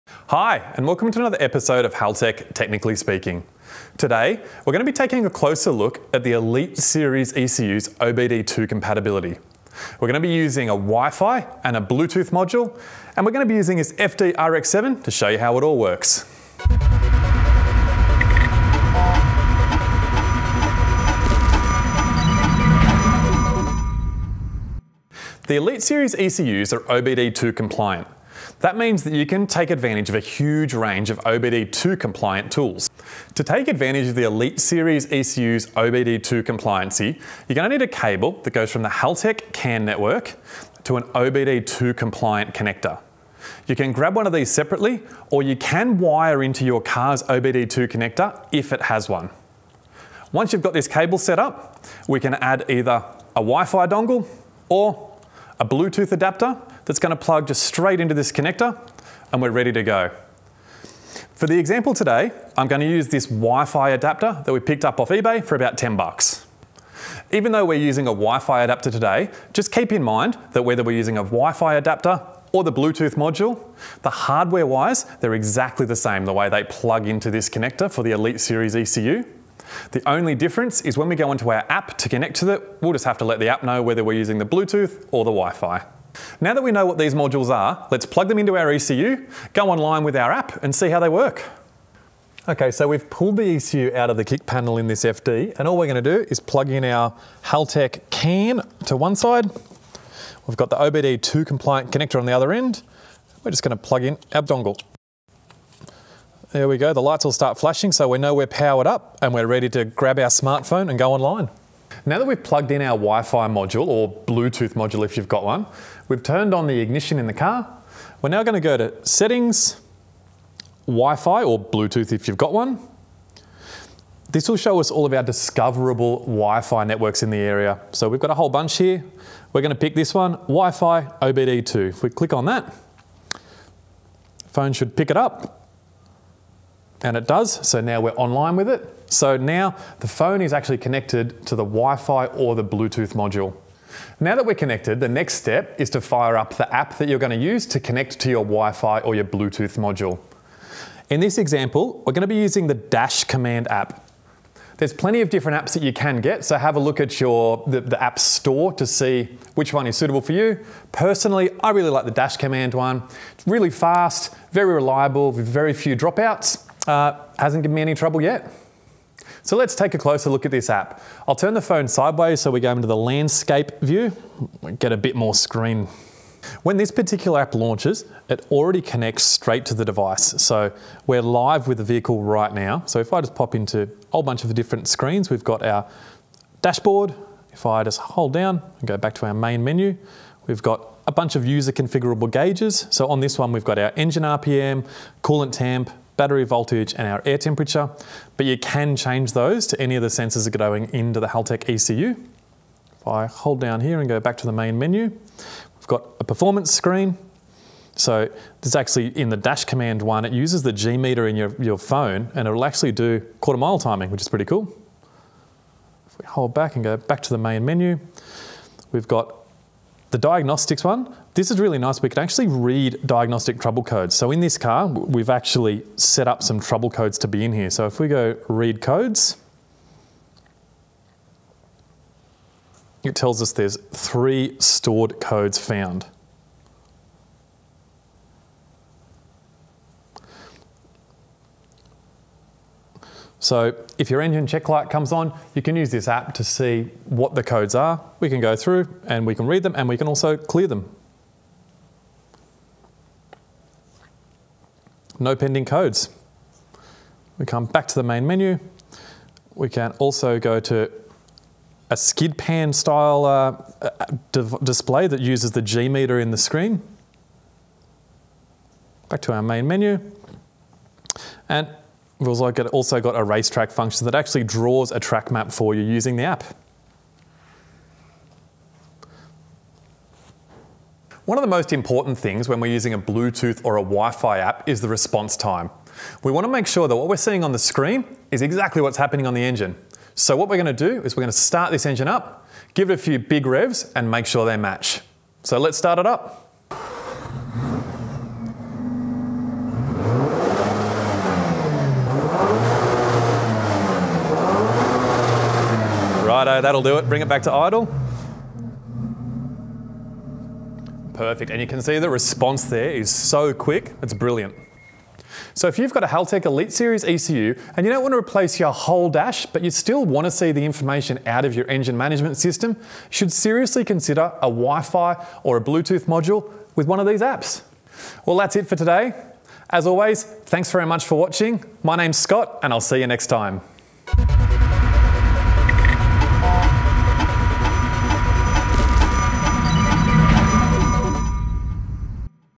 💬 How to use your smart phone with Elite Series ECU - Haltech DIY
All Elite series ECUs are capable of connecting via an OBDII port. This means you can connect a wi-fi or a bluetooth module to to it and use it with mobile devices like your smart phone or a tablet.
In this episode of Technically Speaking we show you, step-by-step, how to connect you smart phone to an Elite Series ECU via a Wi-Fi or Bluetooth module.
For this video we've used a "Dash Command" app. There are many other apps out there, but we we've found this one to be quite rich in features and reliable.

Part No: HT-135000.